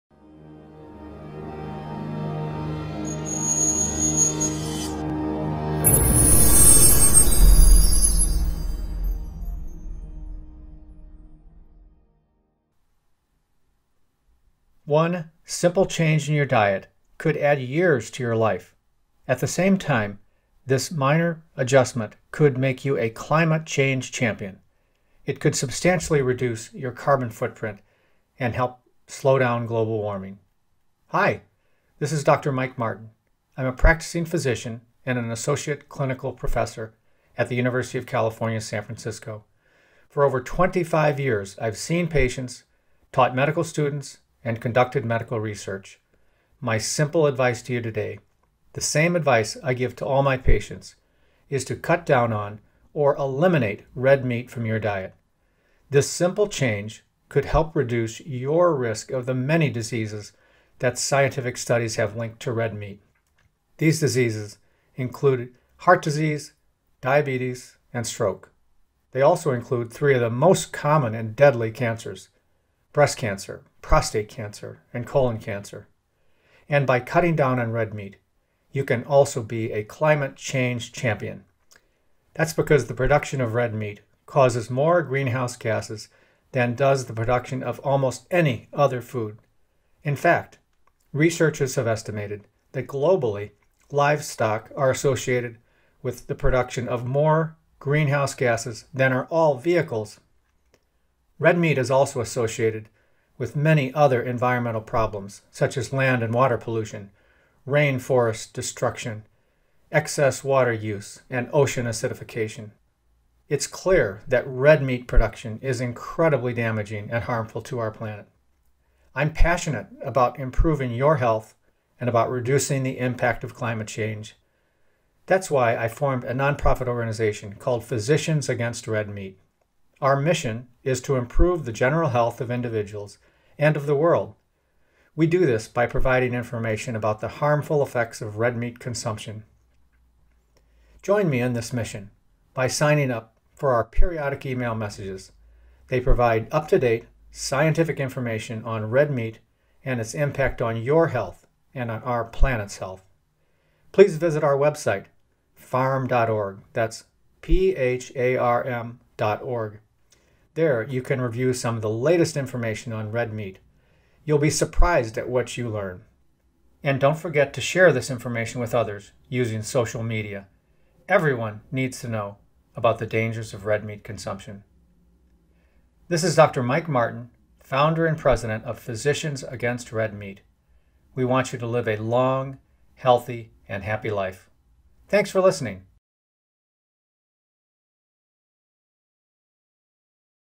Health, Climate Change & Red Meat
Physicians Against Red Meat (PhARM) the adverse health and environmental impact of eating red meat.